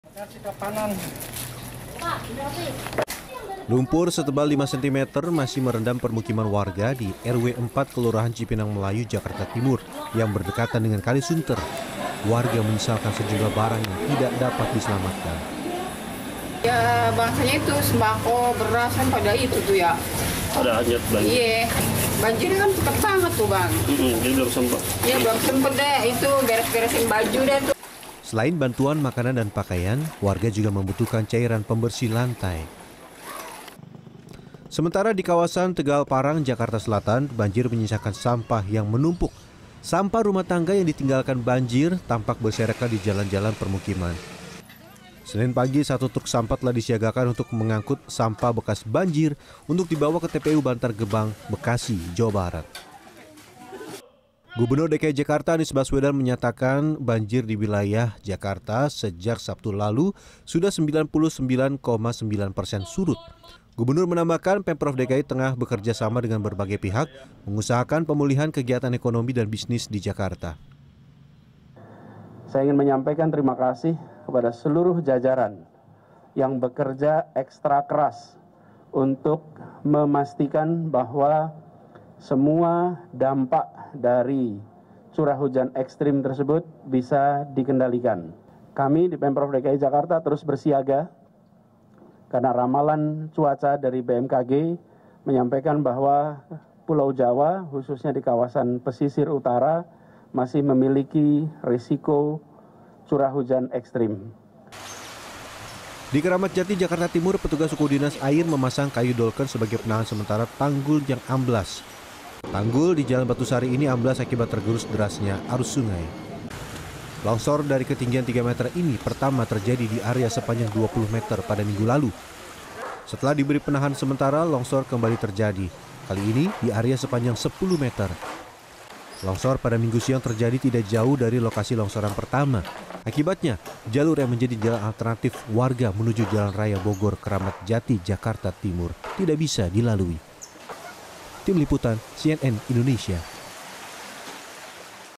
Anies Baswedan Sebut Banjir Jakarta 99,9 Persen Surut
Banjir yang mulai surut dimanfaatkan sejumlah warga ibu kota untuk membersihkan rumah mereka yang kini dipenuhi lumpur. Sementara itu, gubernur DKI Jakarta Anies Baswedan menyebut, banjir telah surut 99 persen. Pemprov kini mengusahakan pemulihan kegiatan ekonomi warga DKI.

Gubernur DKI Jakarta, Anies Baswedan menyatakan, banjir di wilayah jakarta sejak Sabtu lalu sudah 99,9 persen surut.

Gubernur menambahkan, Pemprov DKI tengah bekerja sama dengan berbagai pihak, untuk mengusahakan pemulihan kegiatan ekonomi dan bisnis di Jakarta.

Like & Follow Facebook:
CNN Indonesia

cnnindonesiatv